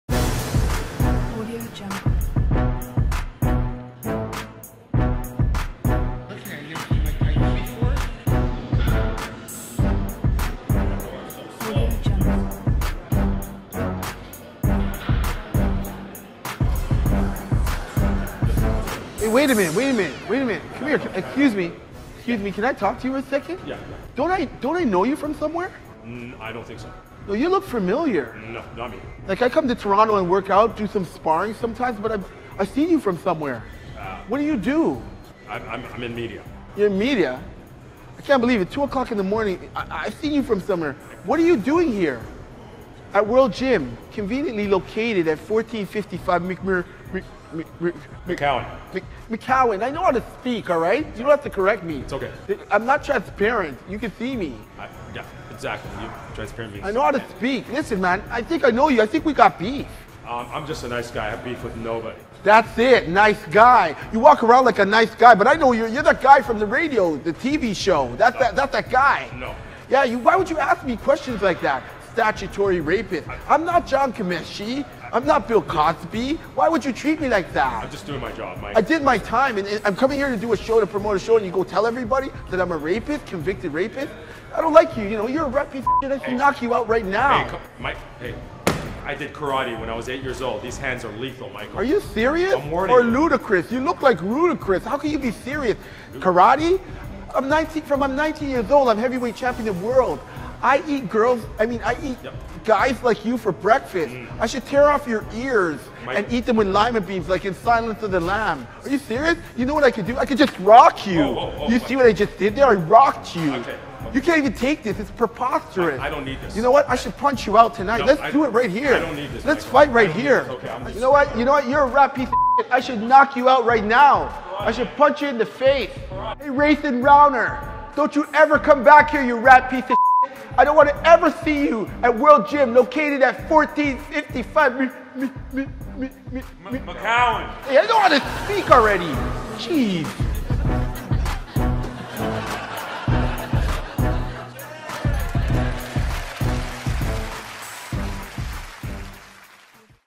MIKE TYSON VS NATHAN DOWNER PT 2
Mike Tyson and Nathan Downer just so happen to have a second encounter a few weeks after their infamous interview.

No tv camera's, just the two of them. its just jokes people!